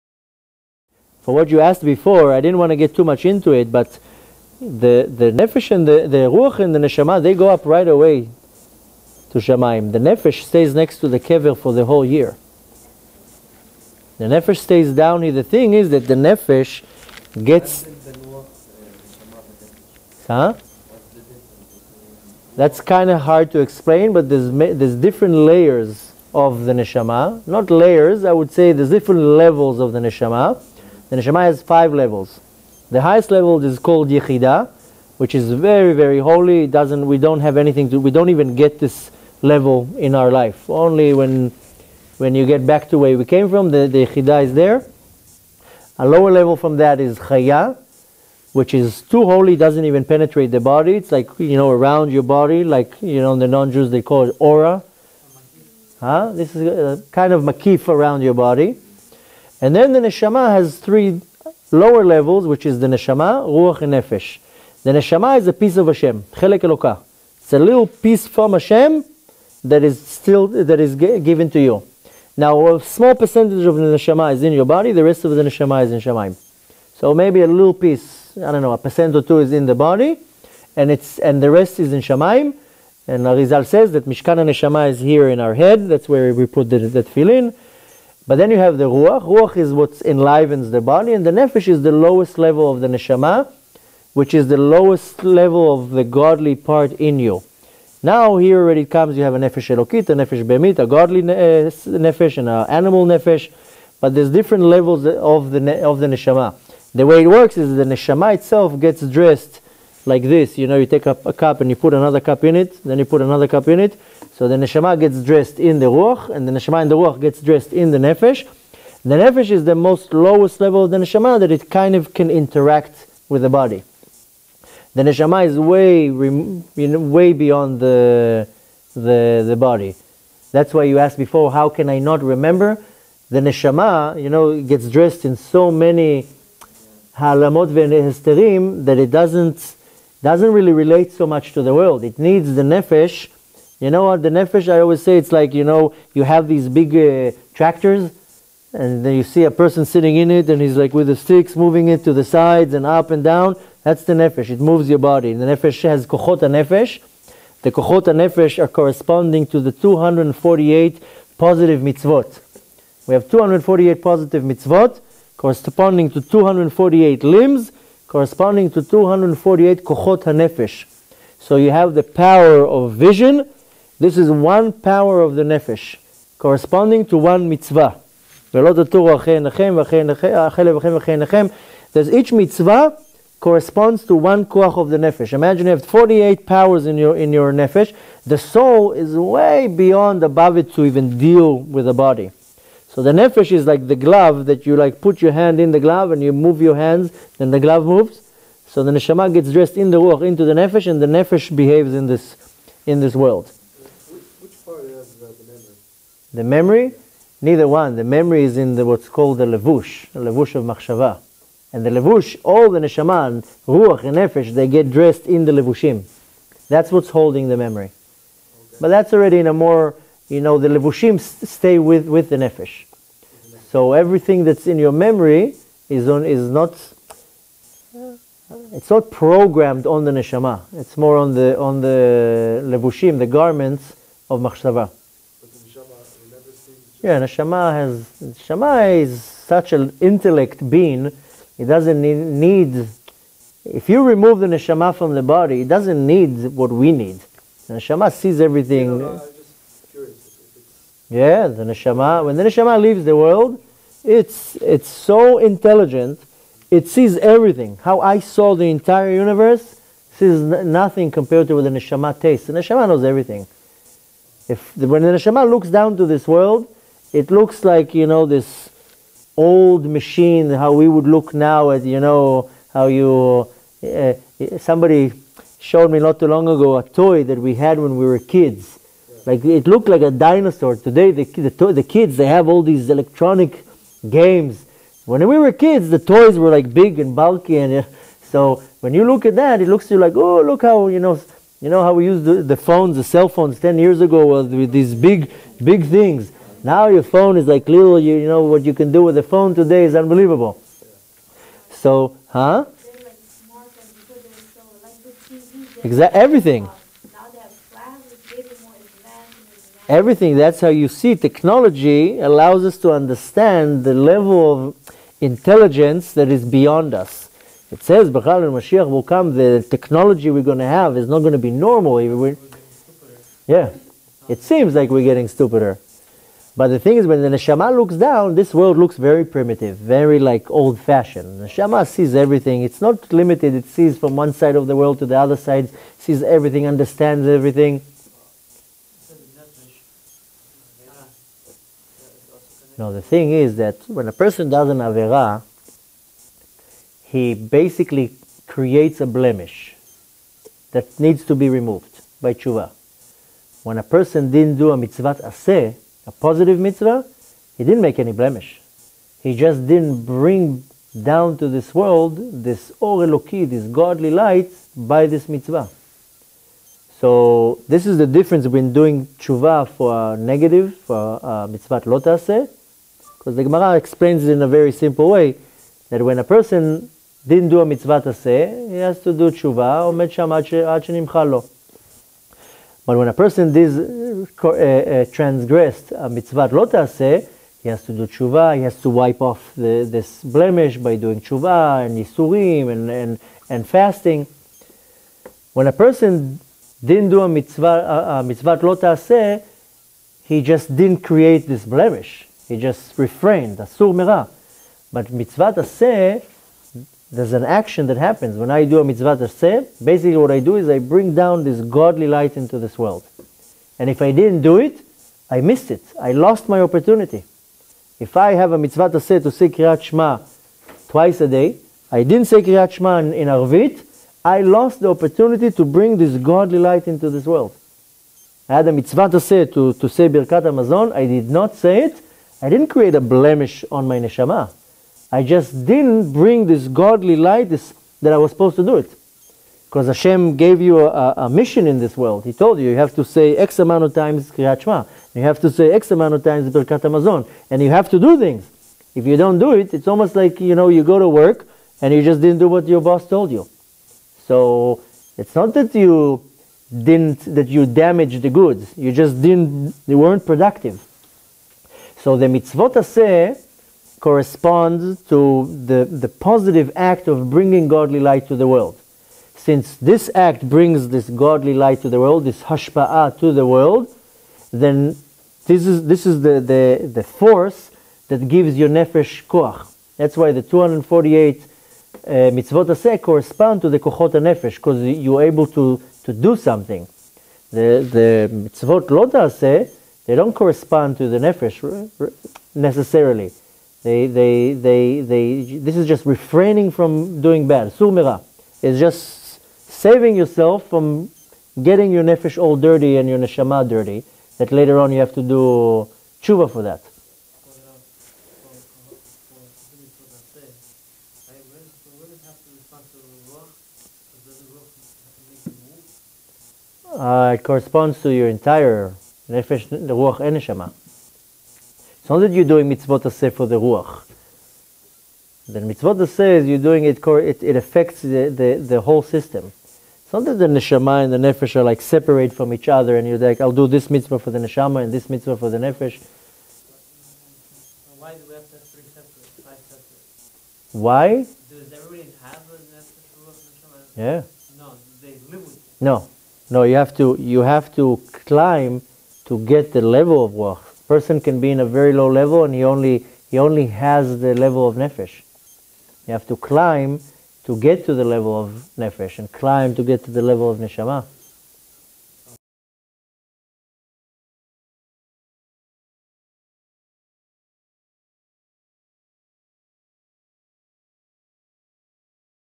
Nefesh Ruach Neshama - Levels of Soul Consciousness and how we effect it with our actions - Part 9
The divine soul involves five ascending levels of consciousness. The divine soul is an indivisible essence inseparably bound to G‑d, as the verse states, "For part of G‑d is His People"

This is part 10 of the questions and answers from a long lecture of Rabbi Alon Anava about his personal near death experience.